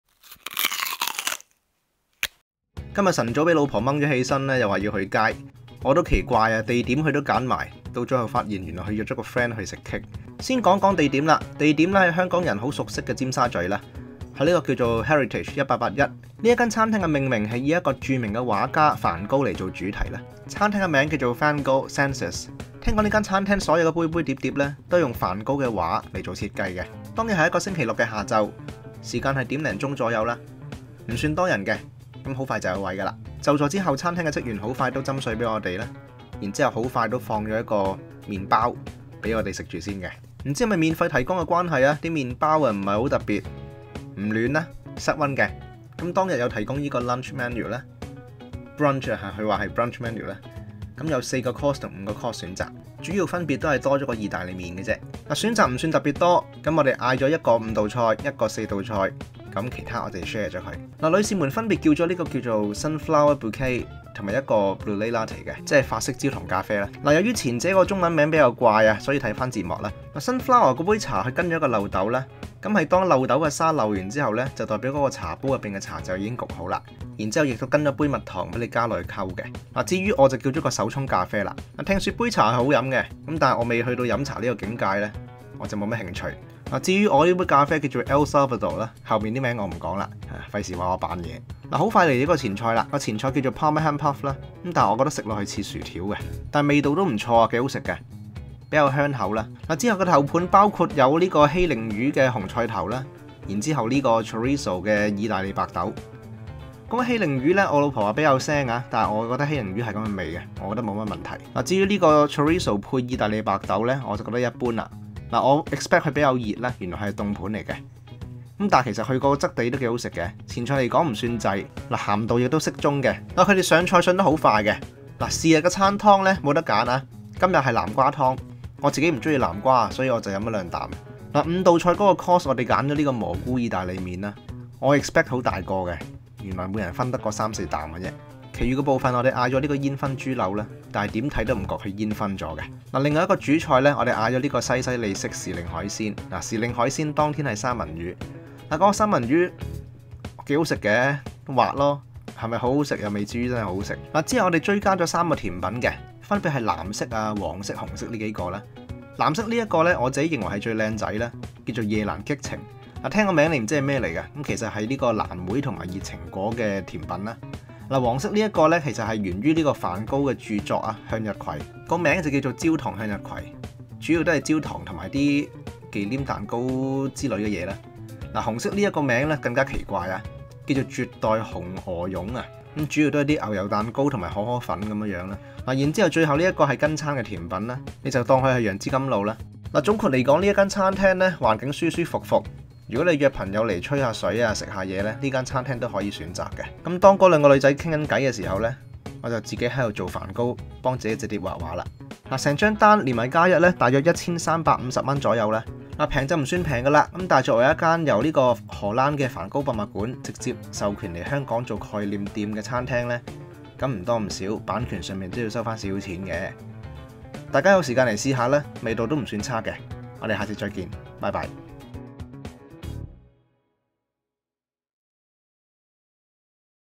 香港梵高 SENSES (VLOG) - Van Gogh SENSES Hong Kong Review
唔太好食甜品嘅我俾老婆的咗去食cake，想知我有咩感受？睇片啦 :) 今次試下做短少少條片，大家比啲意見。

呢間餐廳嘅地址係 香港尖沙咀 Heritage 1881，叫 Van Gogh SENSES.
主要係圍繞著名畫家梵高嘅畫作來做主題，如果各位係藝術派人士，不妨睇睇呢間餐廳將畫作帶到現實嘅心機。
雖然味道我覺得唔算特別出眾，不過都唔算差，不過每件都係心機，無侮辱到藝術品，值得嘉許。

味道：7/10
禮貌：8/10
清潔：8/10
抵食：5/10
上菜速度：8/10